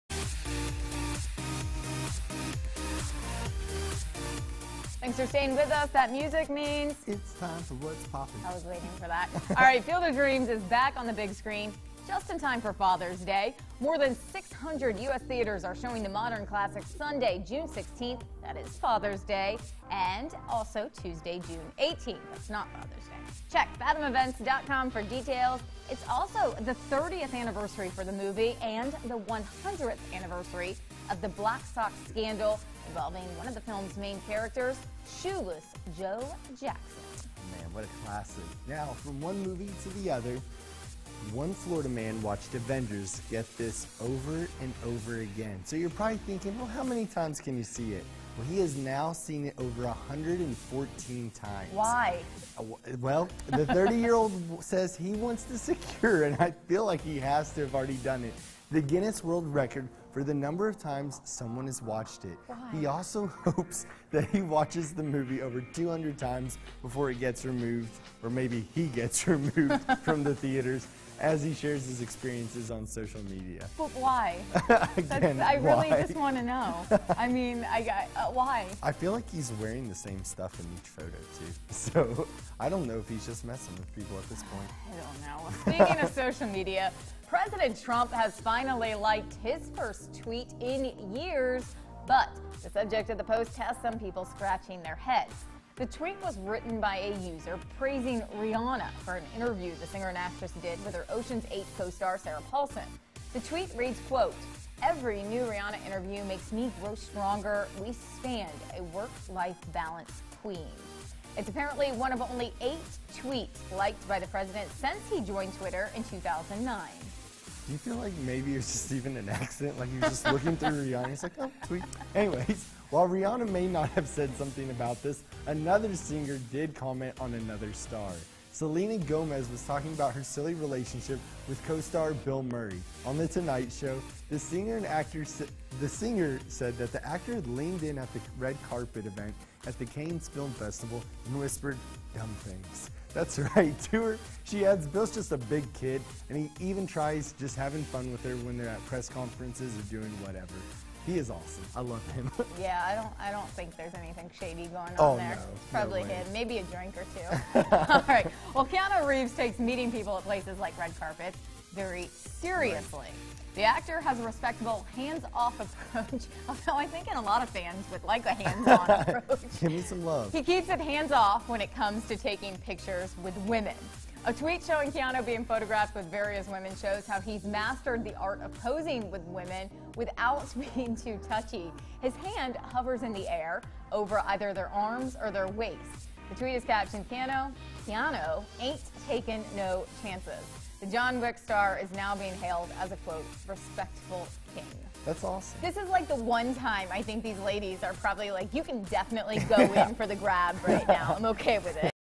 Watching “Endgame” 100+ times, a Father’s Day treat, and more in What’s Poppin’
Topics from today’s What’s Poppin’:
"Field of Dreams" is back on the big screen for Father's Day!
One Florida man watched "avengers; endgame" over and over and over again.
Keanu Reeves has a respectable hands-off approach when it comes to taking pictures with women.